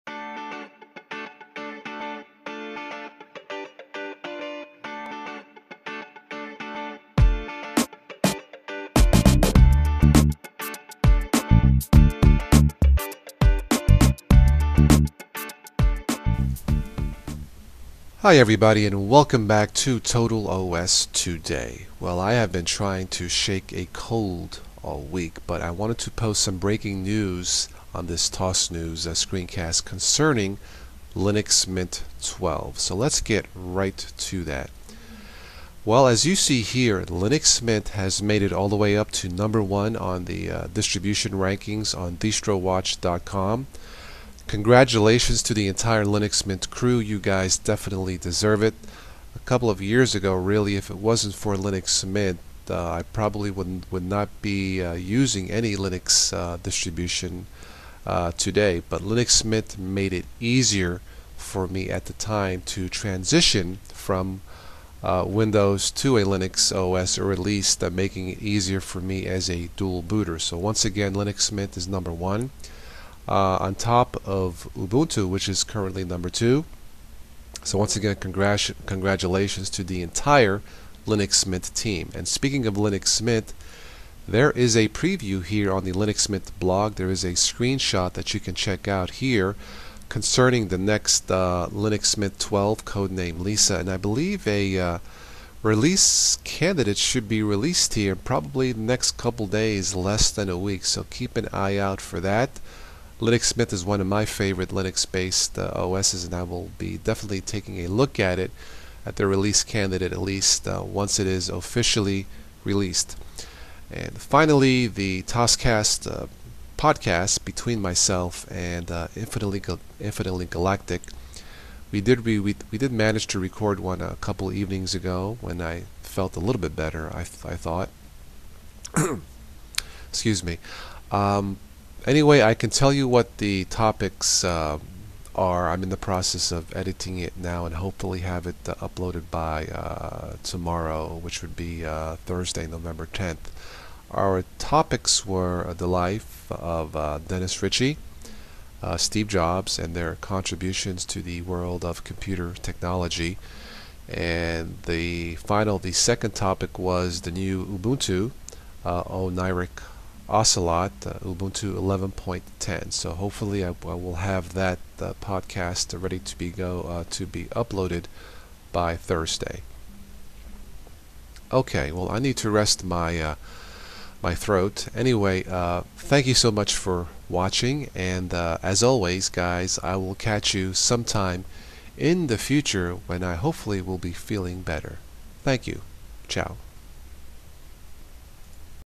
TosNews-Linux Mint #1!! Mint 12, TosCasts 7
Linux Mint currently ranked at the top, Mint 12 preview, TosCasts 7 preview.